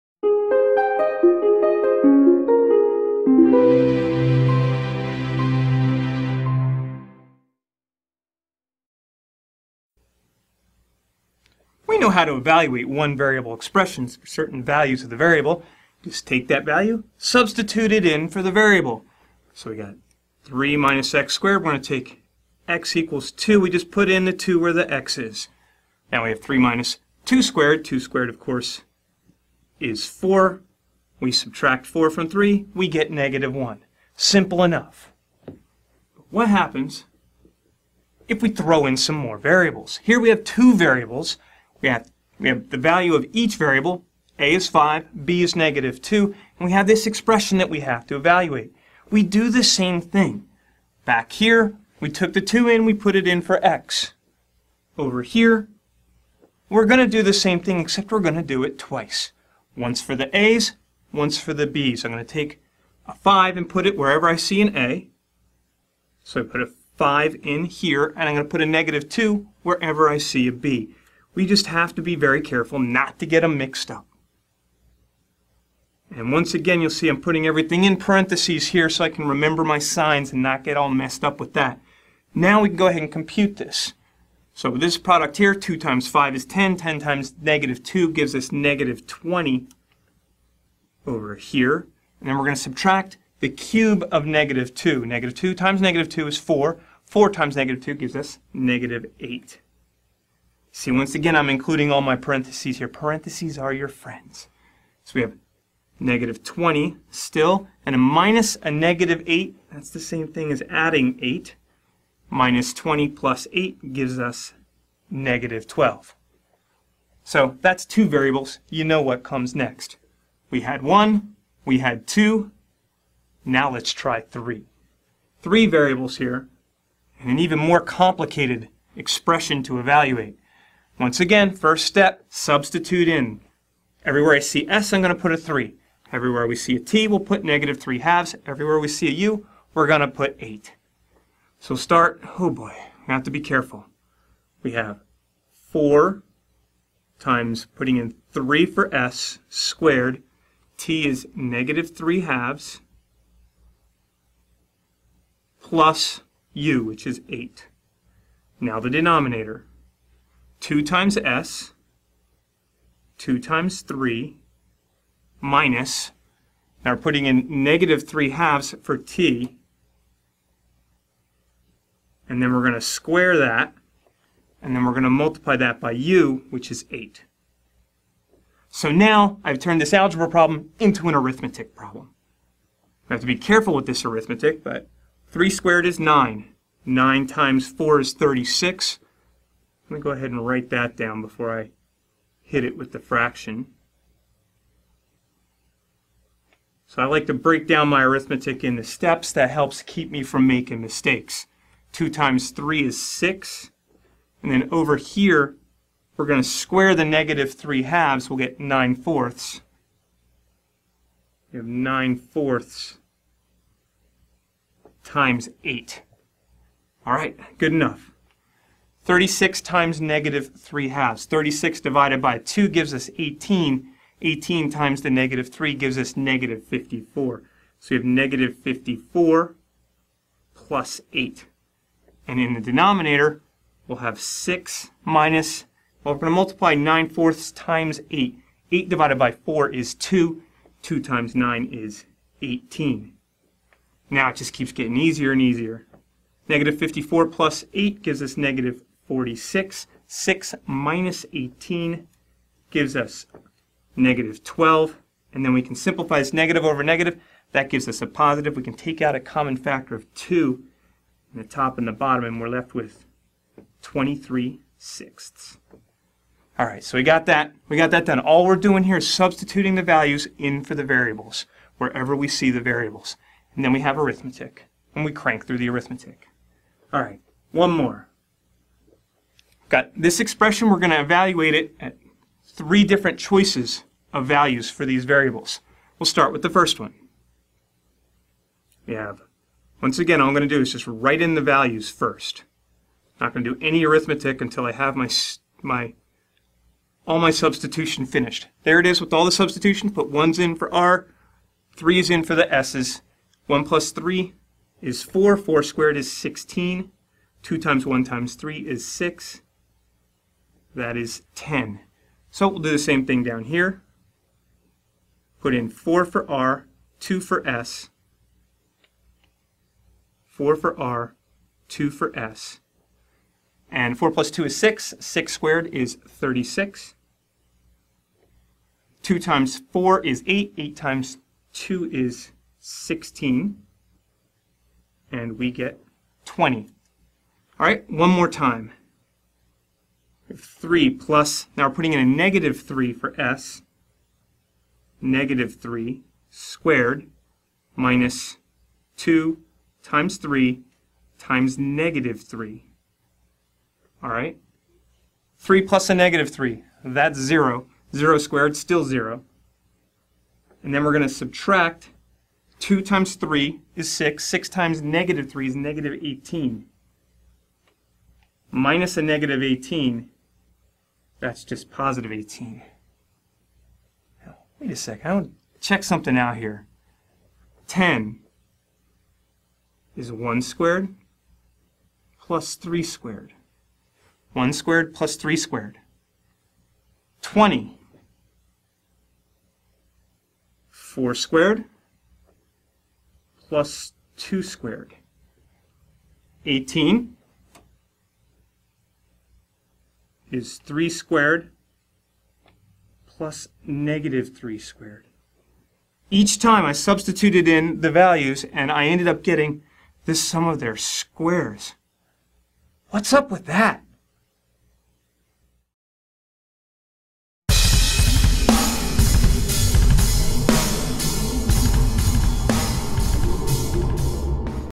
Art of Problem Solving: Evaluating Multi-Variable Expressions
Art of Problem Solving's Richard Rusczyk evaluates expressions with multiple variables.

This video is part of our AoPS Algebra curriculum. Take your math skills to the next level with our Algebra materials: